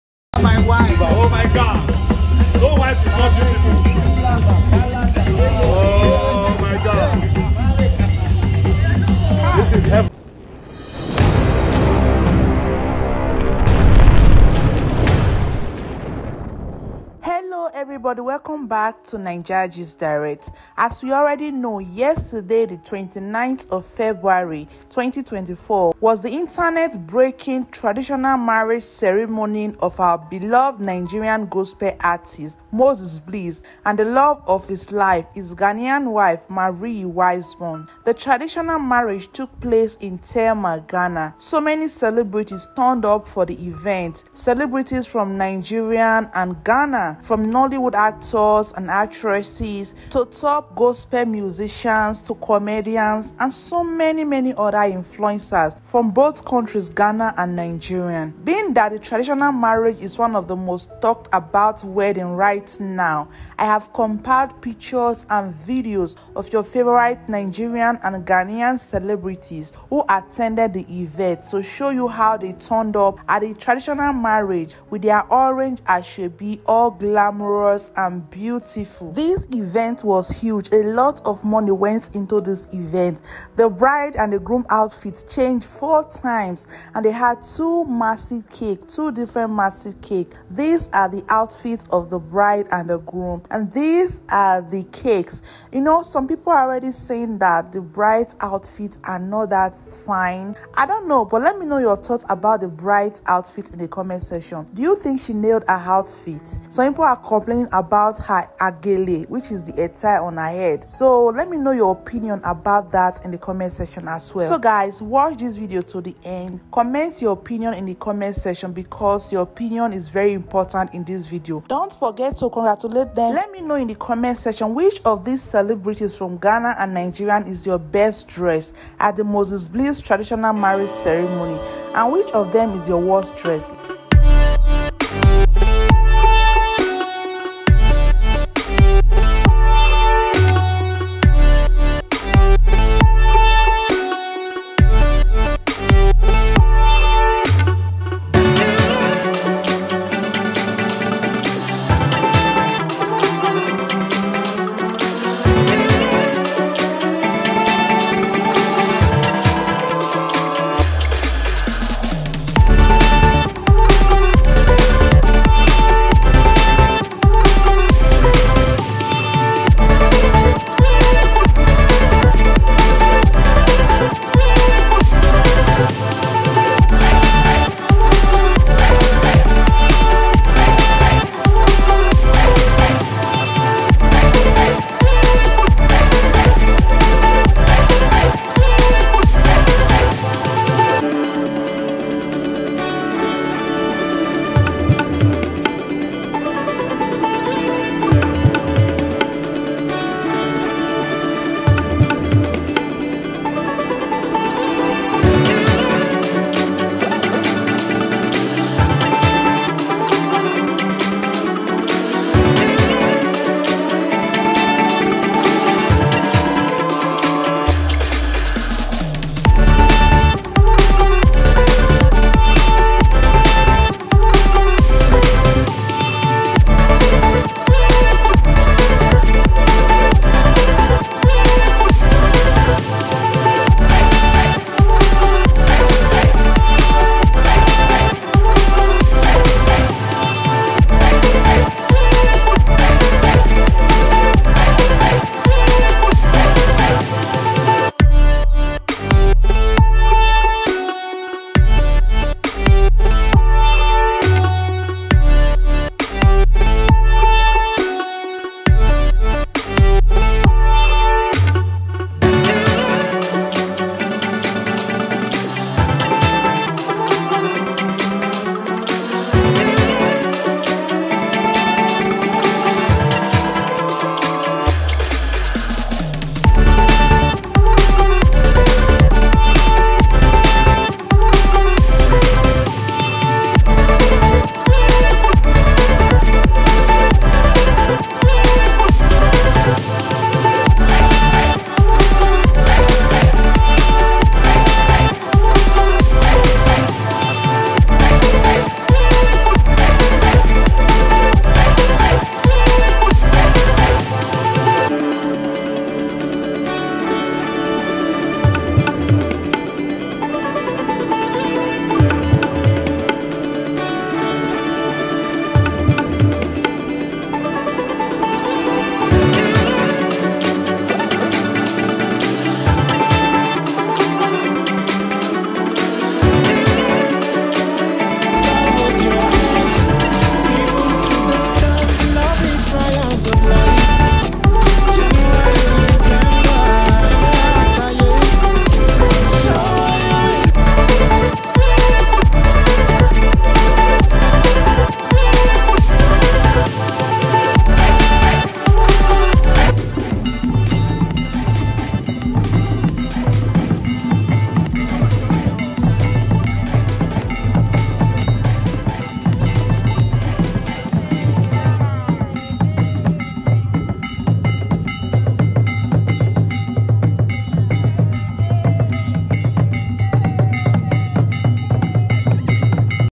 How Nigerian & Ghanaian CELEBRITIES DRESSED For Moses Bliss & Marie Traditional Marriage
Hello everybody welcome back to naija gist direct , as we already know yesterday 29th of February 2024 was the international breaking traditional marriage ceremony of our beloved Nigeria gospel artiste Moses bliss and the love of his life his Ghanaian wife Marie Wiseborn.
The traditional marriage took place in tema Ghana. So many celebrities turned up for the event: celebrities from Nigeria and Ghana.
From nollywood actors and actresses
To top gospel musicians, to comedians and so many other influencers from both countries.
Being the traditional marriage is one of the most talked about right now, I have compiled pictures and videos of your favorite Nigerian and Ghanian celebrities who attended the event to show you how they turned up at the traditional marriage with their orange ashoebi all glamorous.